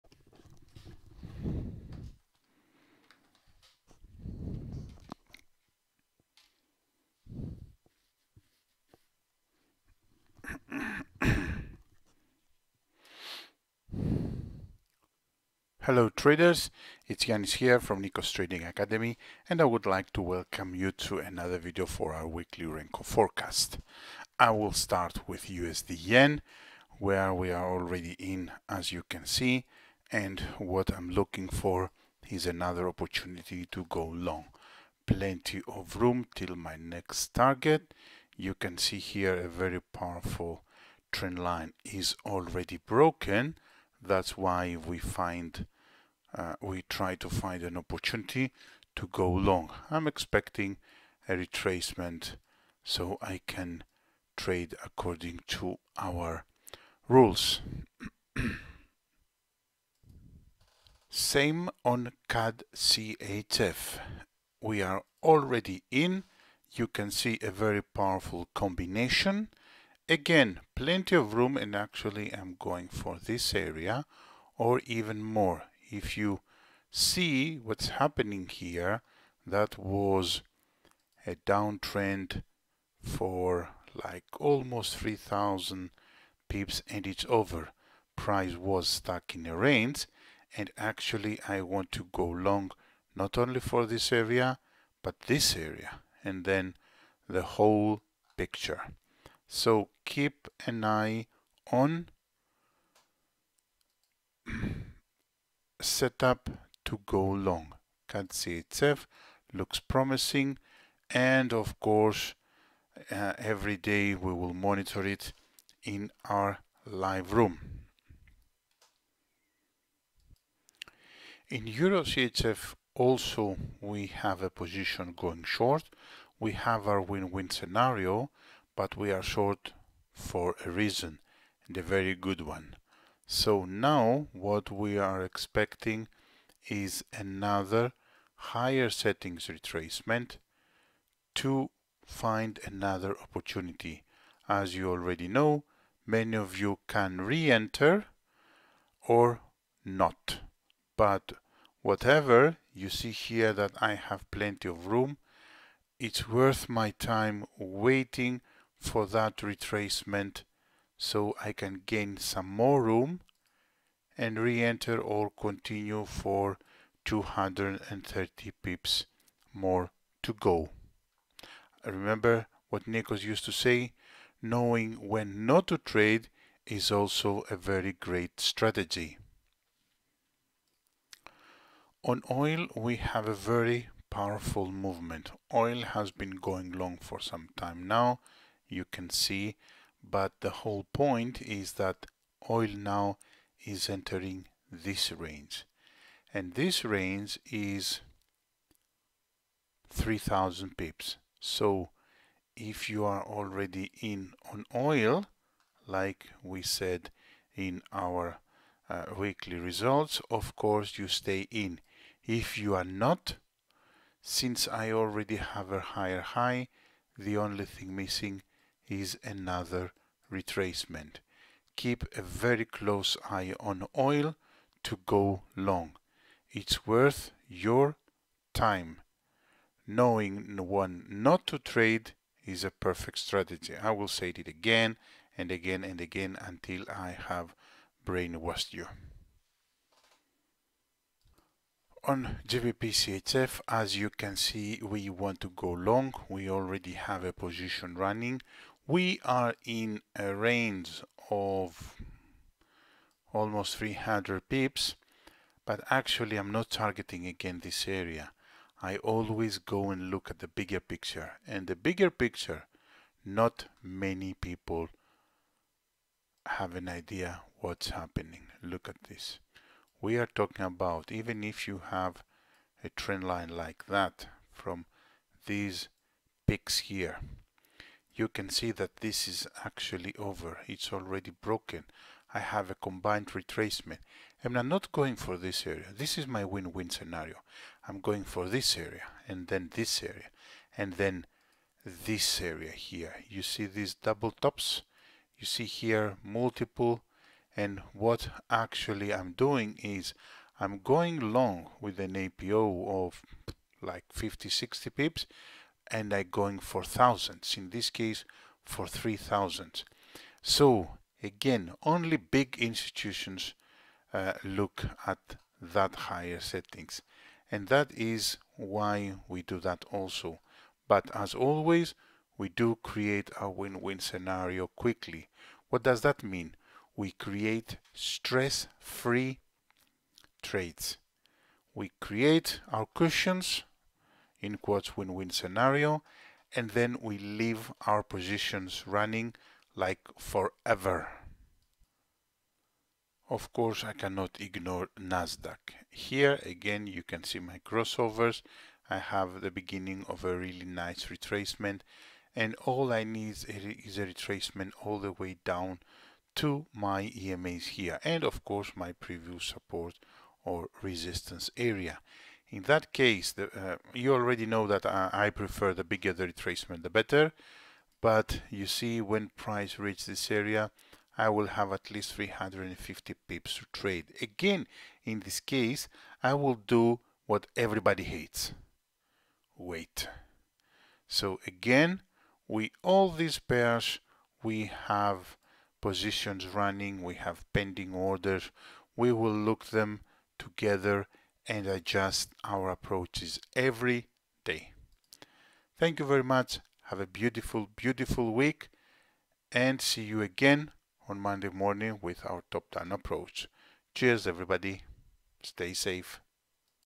Weekly Renko Forecast 11 - 15 Oct 2021
00:00 -  USDJPY
01:00 -  CADCHF
02:01 -  EURCHF
03:01 -  WTI
04:01 - GBPCHF
05:42 - NASDAQ

Enjoy the Weekly Renko Forecast and remember to comment, share and like this video!

Happy & Healthy Trading
Nikos Trading Academy Team